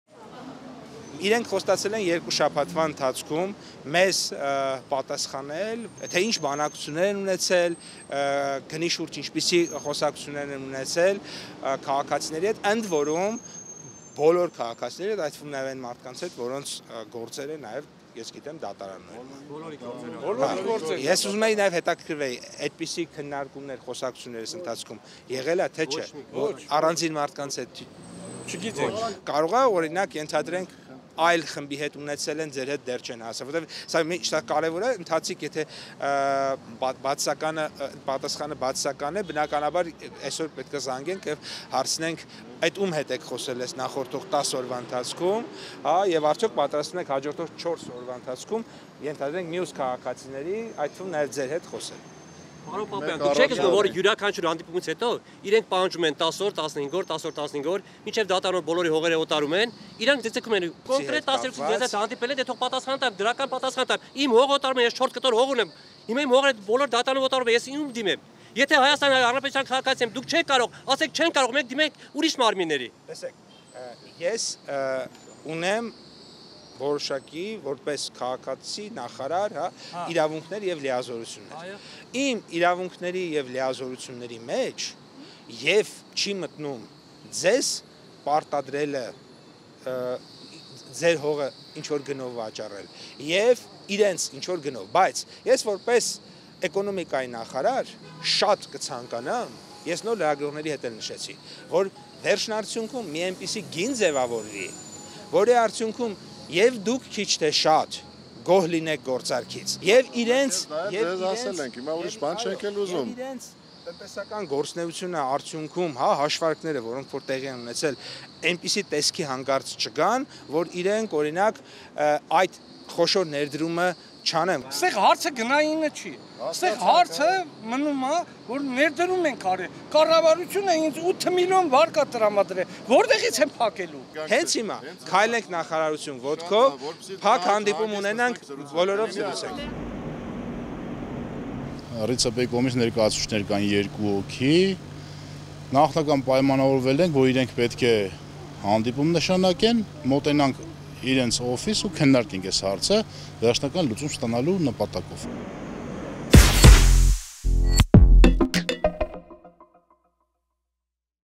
Վարդենուտցիները կրկին բողոքի ակցիայով Երևանում են․ իրենց հարցերին վերջնական լուծում են ակնկալում
Գյուղացիների հողերը կառավարությունը հանրային գերակա շահ է ճանաչել, իսկ «Ռիդ» ընկերությունը, որը տարածքում մեծ ներդրումային ծրագիր է սկսում, հողերն ուզում է ստանալ, ըստ գյուղացիների, կոպեկներով՝ մեկ քառակուսի մետրի դիմաց 345 դրամ է առաջարկում։